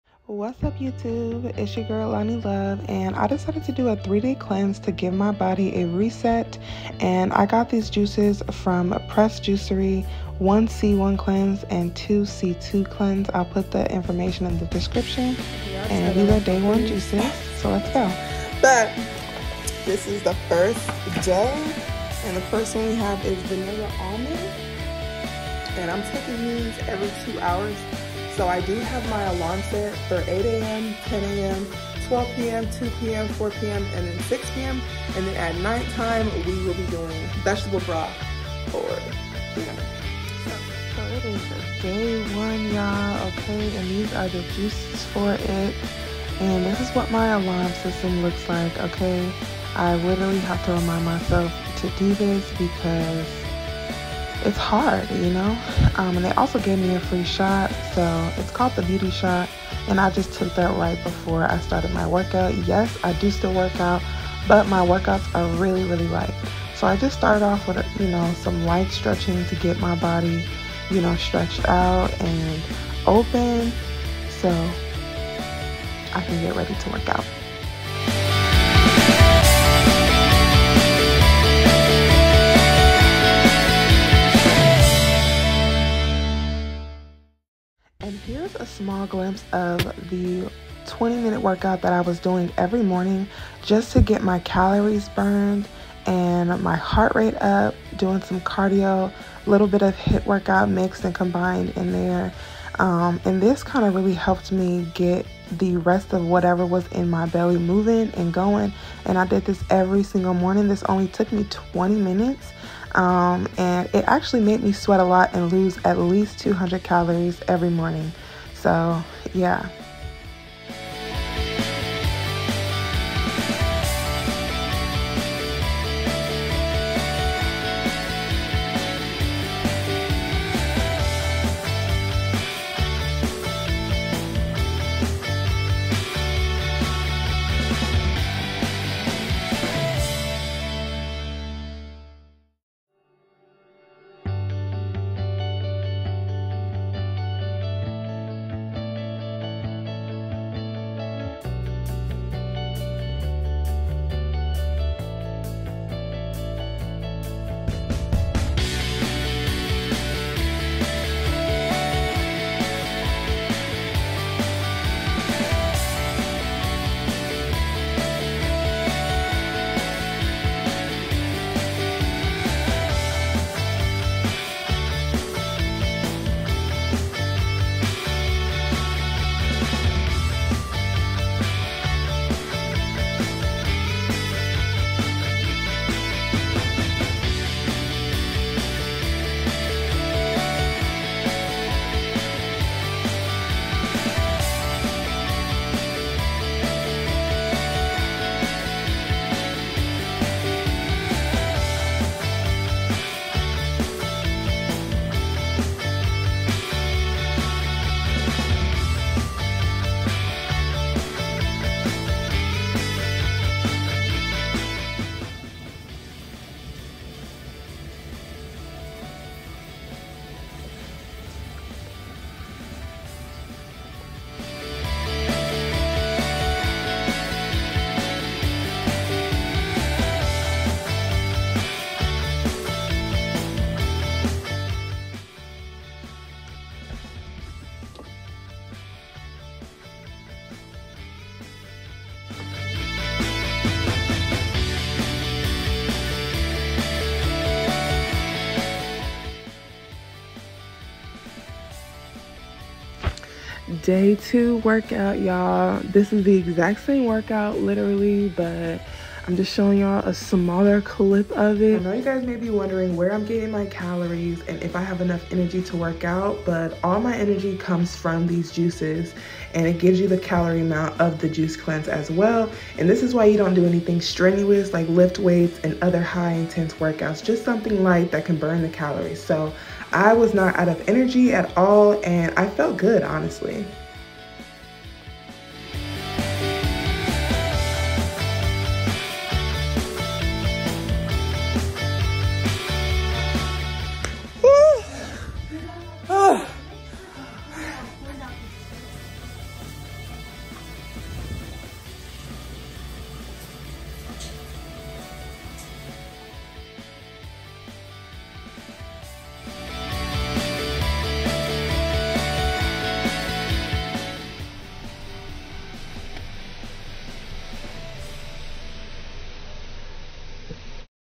Lost 8Lbs | 3 Day Fast Juice Cleanse
Day 1- Cleanse 1 Pressed juicery
Day 2- Cleanse 2 Pressed juicery
Day 3-Cleanse 3 Pressed juicery ⬇️⬇️⬇️

Editing: IMovie / Canva